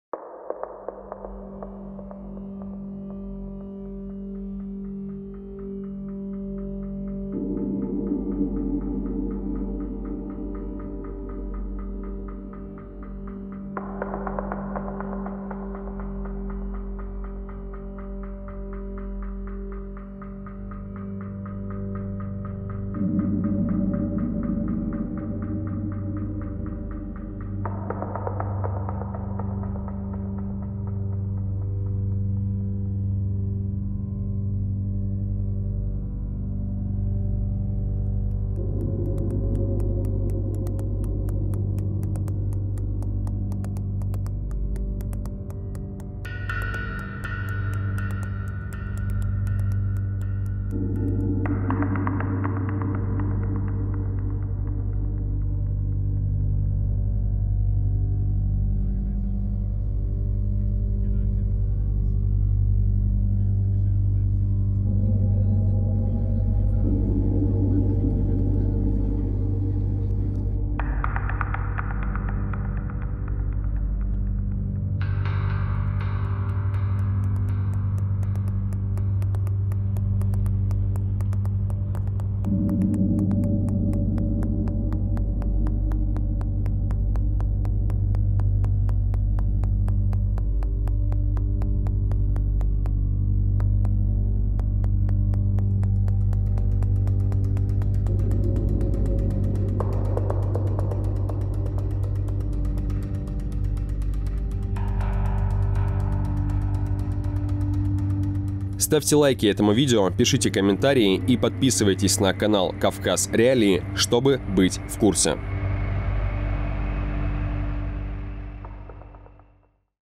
Рейды кадыровцев против символики Ичкерии
Жители Чечни сообщают о рейдах силовиков, которые ищут на одежде и личных вещах символику волка. Это хищное животное занимает особое место в культуре чеченцев, символизируя мужество и стойкость, силуэт волка был гербом и изображался на флаге непризнанной Чеченской республики Ичкерии.